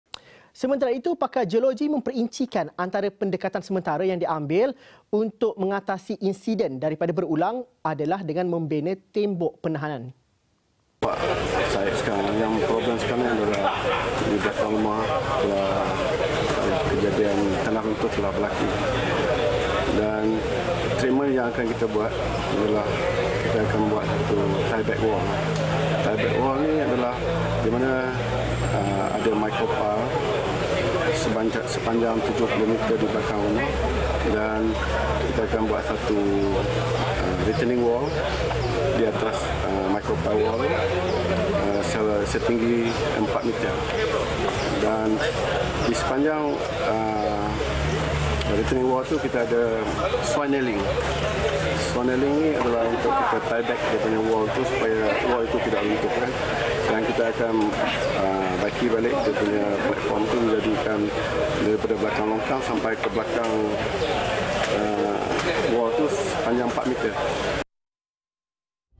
Bina tembok penahanan elak runtuhan berulang
Pakar geologi memperincikan antara pendekatan sementara yang diambil untuk mengatasi insiden daripada berulang adalah membina tembok penahan.